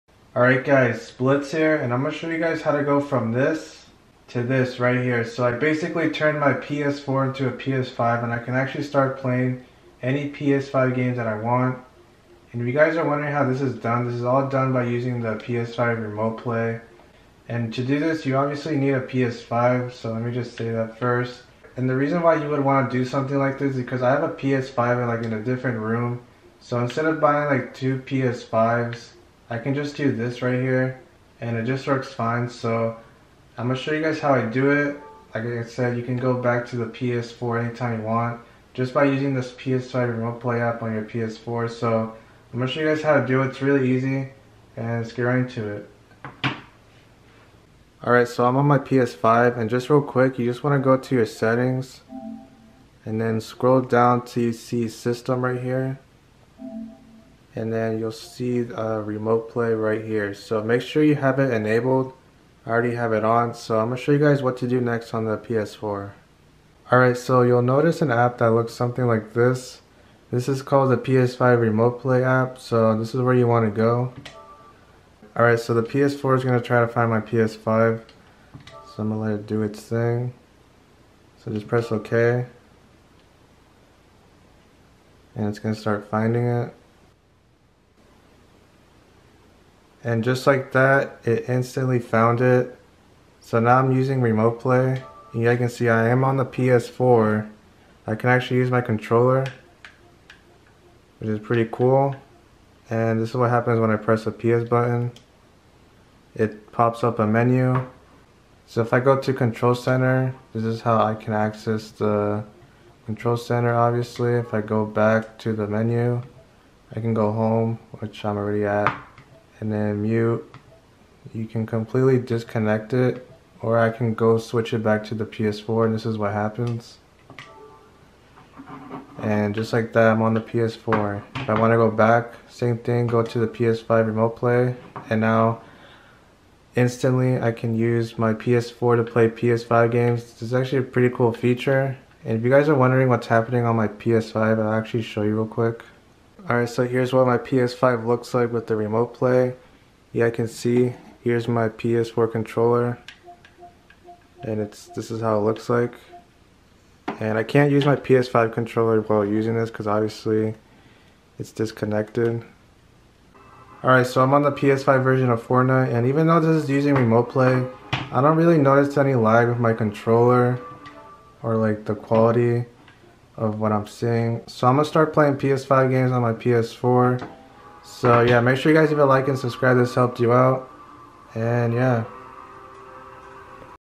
How to PLAY PS5 GAMES ON PS4! (PS5 Remote Play) (EASY METHOD)
You can now turn your ps4 into a ps5 lol.